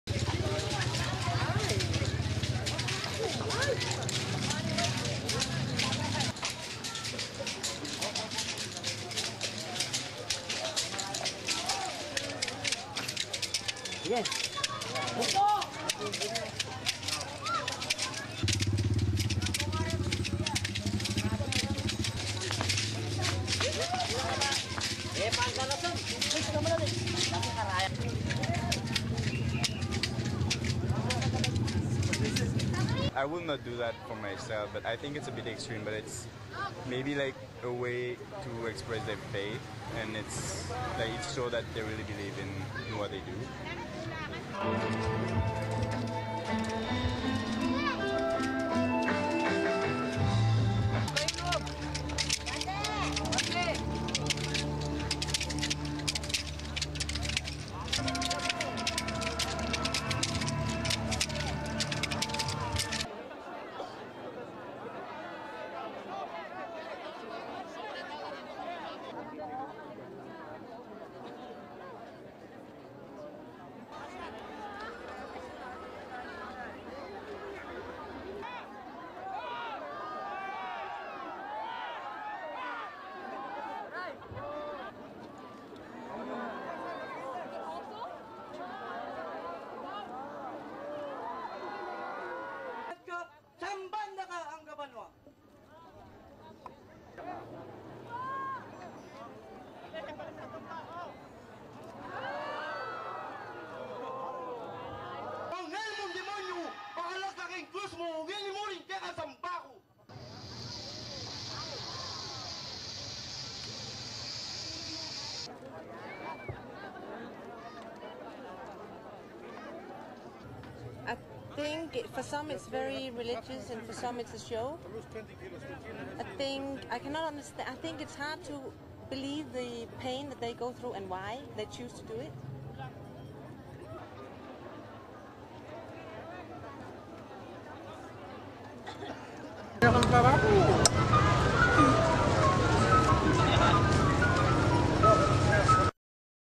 WRAP Good Friday re-enactment of Christ's crucifixion
1. Various of Catholic penitents lashing themselves on the back with bamboo sticks while walking
2. Man putting putting incisions on the back of one of the penitents who then joins the march
3. Various of the march as penitents whip themselves with bamboo sticks
4. Mid of crowd watching as penitents pass by
5. SOUNDBITE: (English) Laurence Gueguen, French tourist:
"I would not do that for myself, I think it's a bit extreme, but maybe it's a way to express their faith and it shows that they really believe in what they do."
6. Penitents on the ground as they have their backs whipped with bamboo sticks
7. Various of penitents walking along whipping themselves
8. Various of penitents dressed as Roman soldiers walking on to mound with three crucifixes erected
9. Cutaway of crowd
10. Mid of penitent Ruben Enaje lying in pain as a nail is being driven into his hand
11. Wide of penitents erecting Enjae on the cross
12. Close-up of Enaje's face
13. Penitent putting nail in Enaje's foot
14. Close up of Enaje's left hand with nail
15. Mid of penitent dressed as soldier pulling nail from hand
16. Wide of scene
17. SOUNDBITE: (English) Susan Kelvian, Tourist, Vox pop:
"I think for some its very religious, and for some a show. I think it is hard to believe the pain they go through and why they choose to do it."
18. Enaje being carried away on stretcher
19. Another penitent's right hand being nailed to the cross
STORYLINE
Philippine devotees re-enacted Jesus Christ's suffering on Friday by having themselves nailed to crosses in rites frowned upon by church leaders in Asia's largest predominantly Roman Catholic nation.
Ruben Enaje was among some 30 men scheduled to go through the Good Friday rites in three villages in northern Pampanga province's San Fernando city.
AP Television pictures showed the moment when one of the nails was driven into Enaje's hand, before the cross was lifted to stand on a dusty mound in San Fernando's San Pedro Cutud village.
Five other devotees, including a woman, were nailed to crosses in nearby Bulacan province.
The yearly tradition has become a tourist attraction, especially in San Pedro Cutud village, which sometimes draws thousands of local and foreign tourists.
Aside from the cross nailings, scores of men pound their bleeding bare backs with bamboo sticks dangling from ropes in a flagellation rite meant to atone for sins.
French tourist Laurence Gueguen, in San Pedro Cutud for the spectacle, said "I would not do that for myself, I think it's a bit extreme, but maybe it's a way to express their faith and it shows that they really believe in what they do."
"I think for some its very religious, and for some a show," said another tourist, Susan Kelvian. "I think it is hard to believe the pain they go through and why they choose to do it."
According to the Christian faith Jesus Christ was crucified on this day.
Good Friday marks the beginning of the Christian festival of Easter.